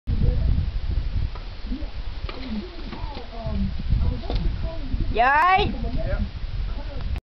tree attack
getting a limb off the trailer next door.